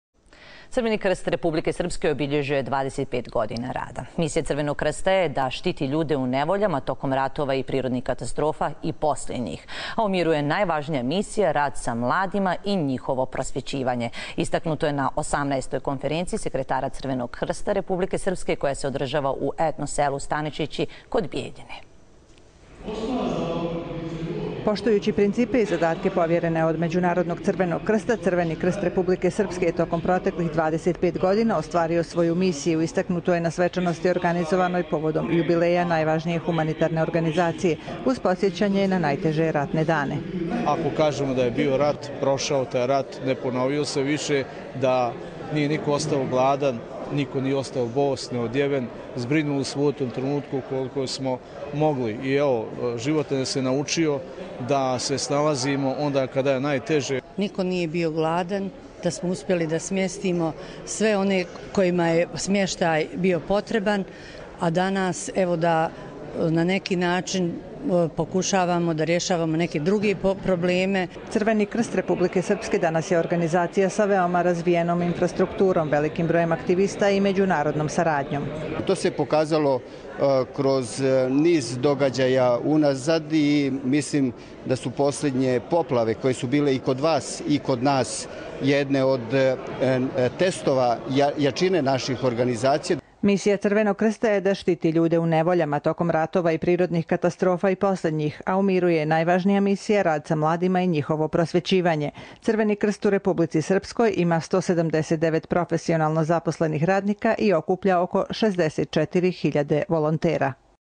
Crveni krst Republike Srpske obilježio 25 godina rada
Crveni krst Republike Srpske obilježio je 25 godina rada. Misija Crvenog krsta je da štiti ljude u nevoljama tokom ratova i prirodnih katastrofa i poslije njih, a u miru je najvažnija misija rad sa mladima i njihovo prosvjećivanje, istaknuto je na 18. konferenciji sekretara Crvenog krsta Republike Srpske koja se održava u Etno selu Stanišići kod Bijeljine.